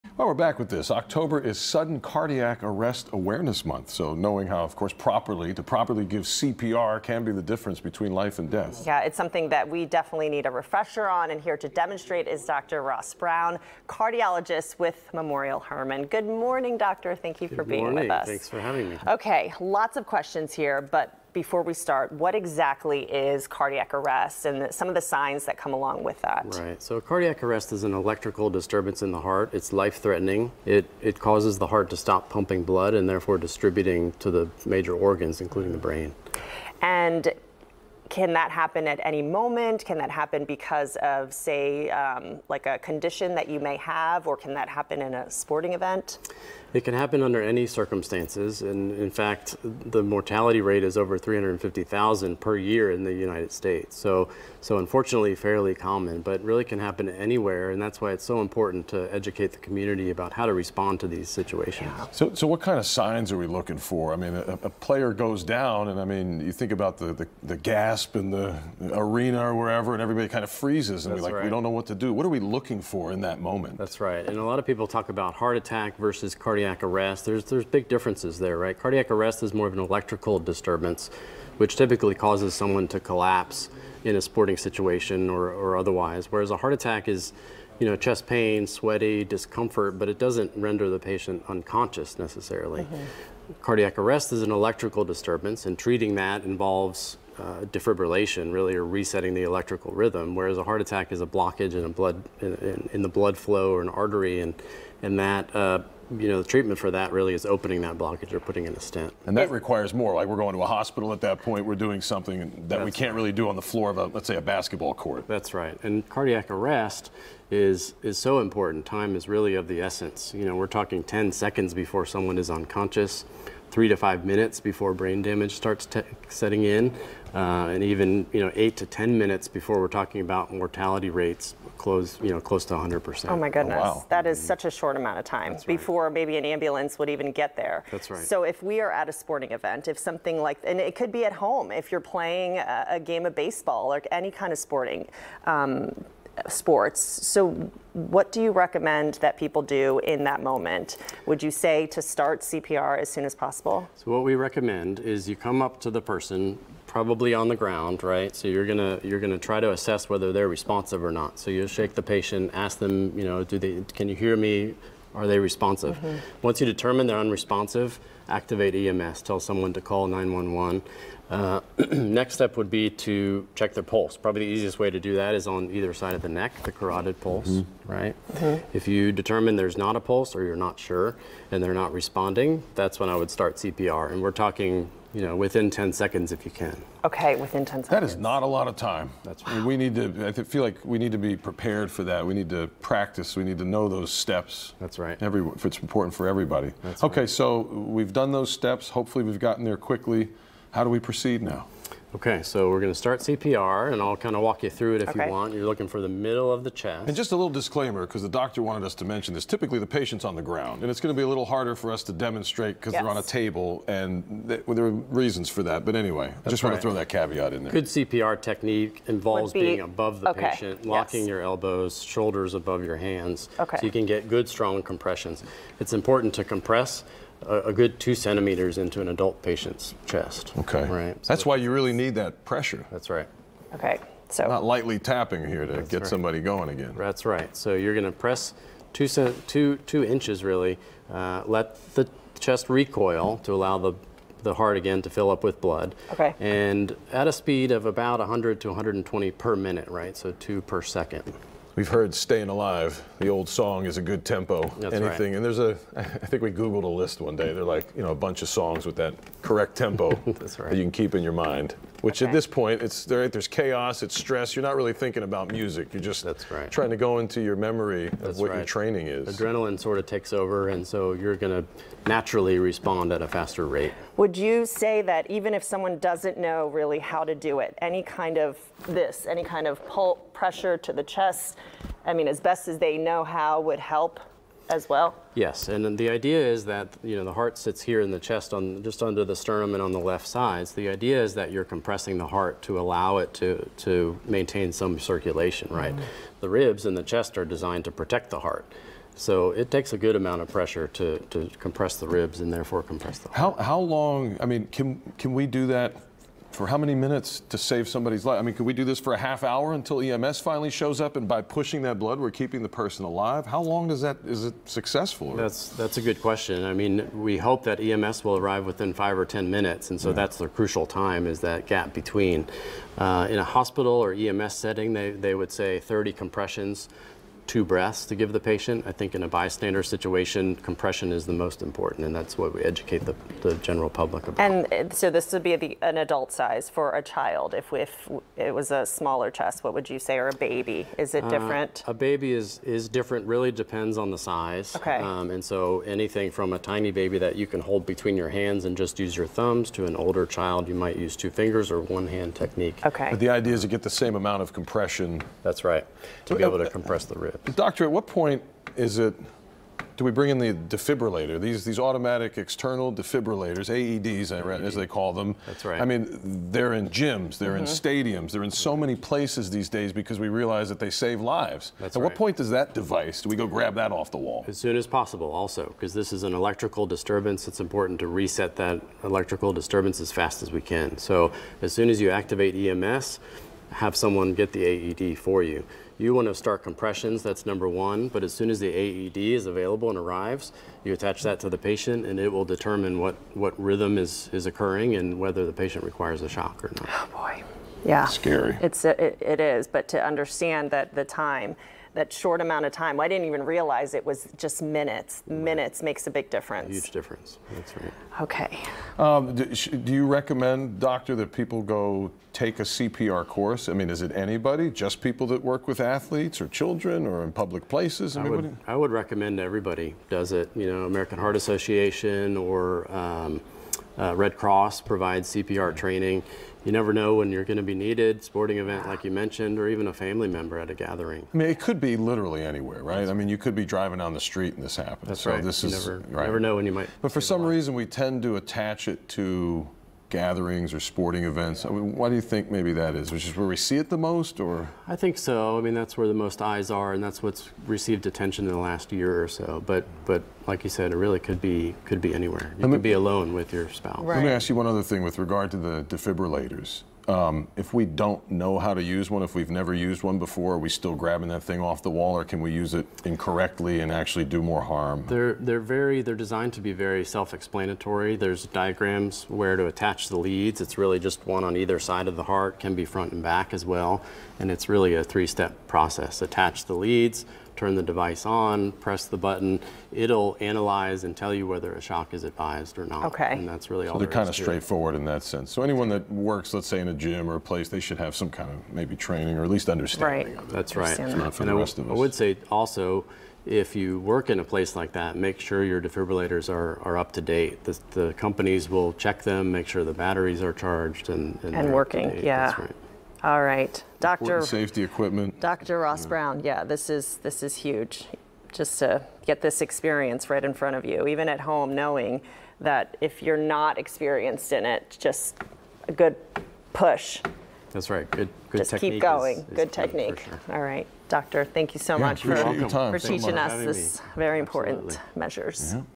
Owen and Sofia get a lesson on how to perform CPR
Dr. W. Ross Brown, cardiologist with Memorial Hermann, gives an in-studio demonstration on how to properly perform CPR.